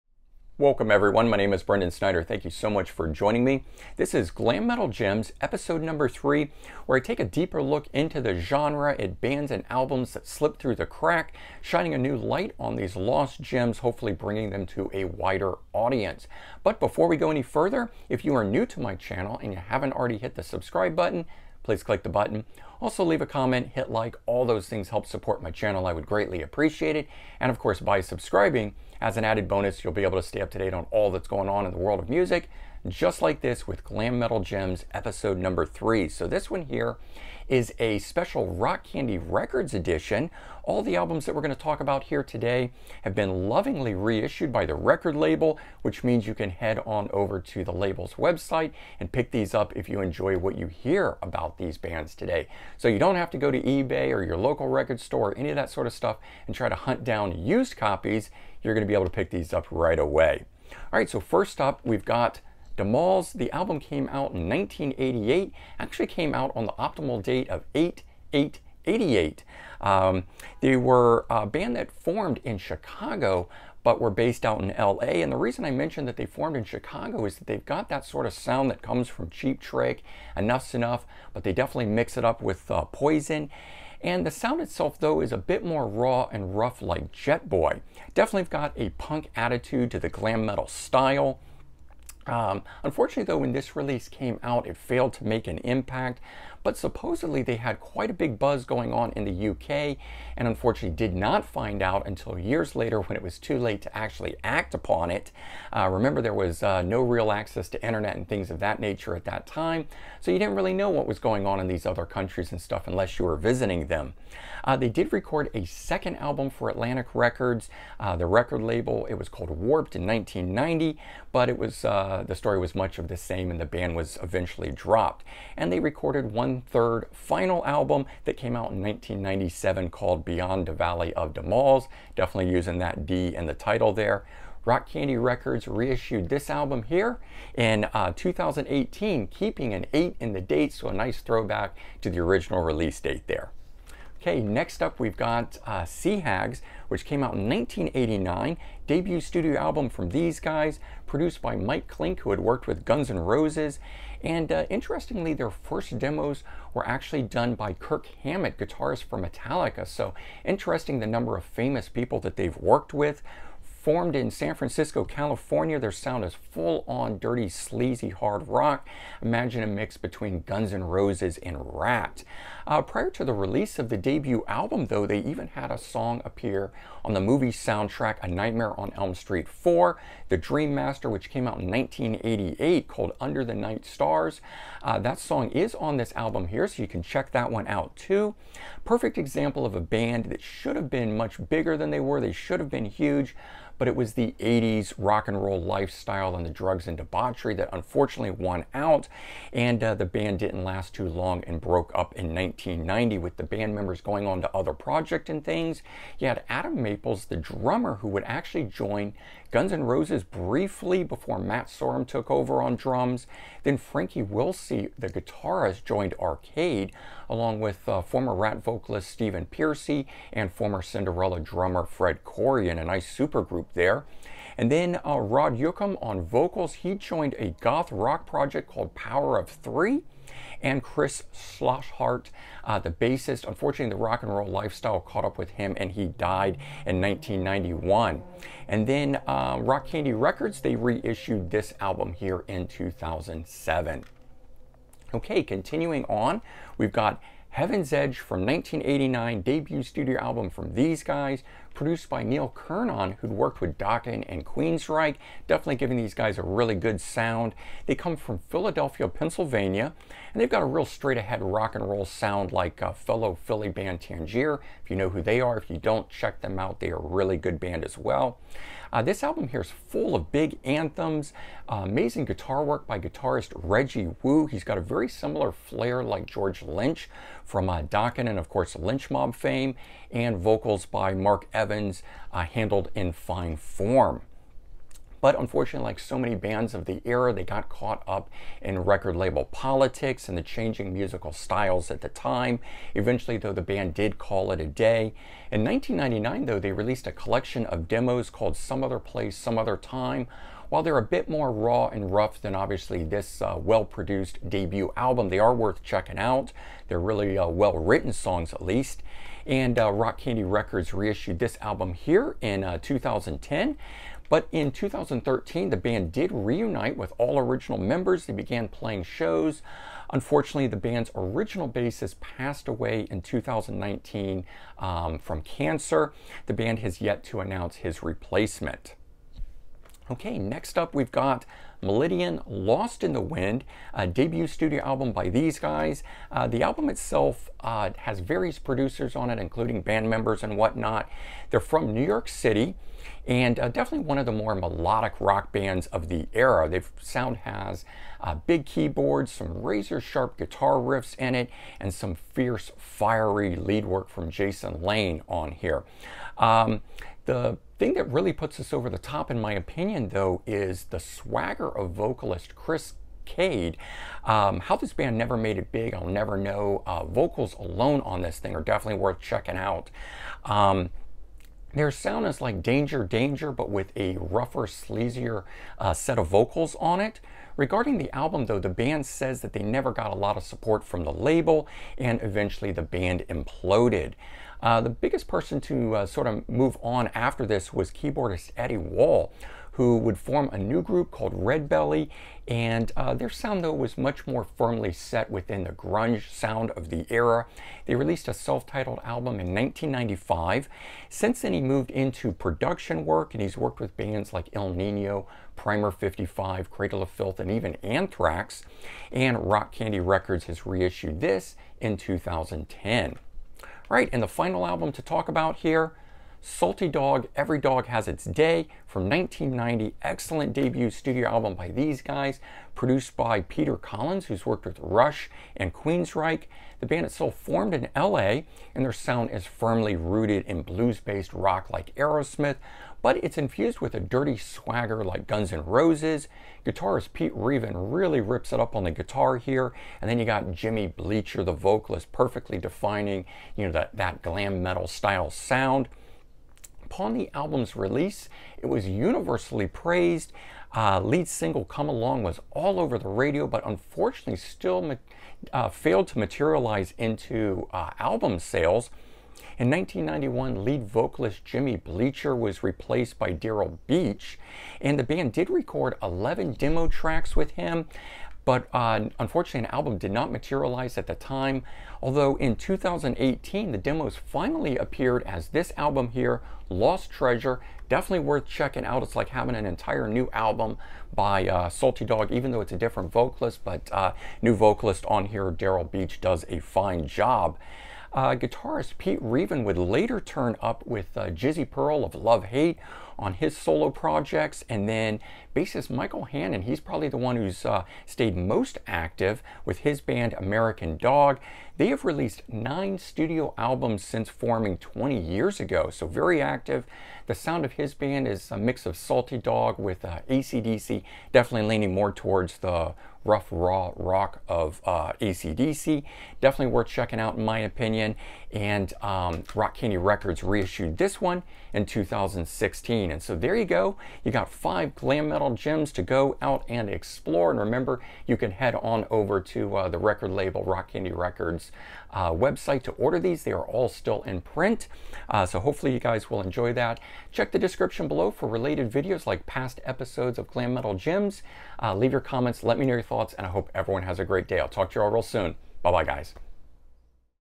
Glam Metal Gems #3 - Rock Candy Records
This series takes a deeper look into the Glam Metal genre at bands and albums that slipped through the cracks.  With the notion of shinning a light on these lost gems, hopefully bringing them to a wider audience.